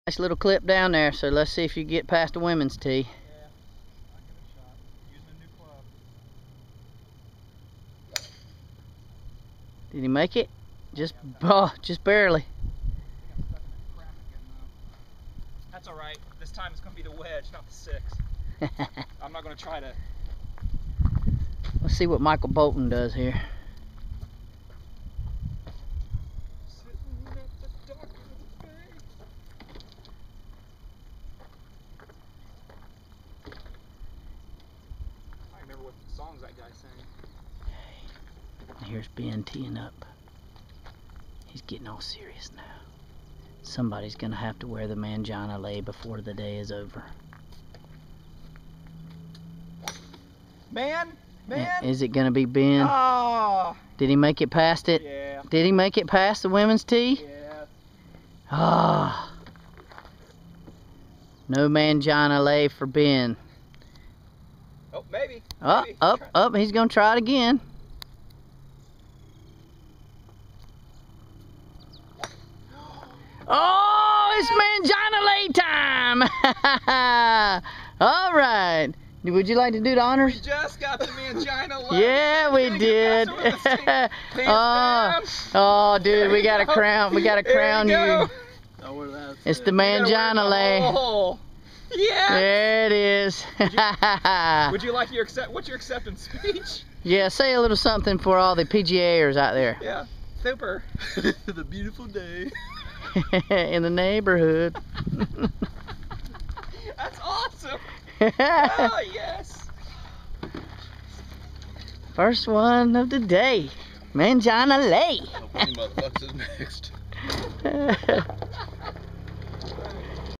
Ben gets the Manginalei
So we have this rule that if you can't hit it past the ladies tee you have to wear the "Manginalei." Ben was the first one to wear it. At least he was a good sport about it.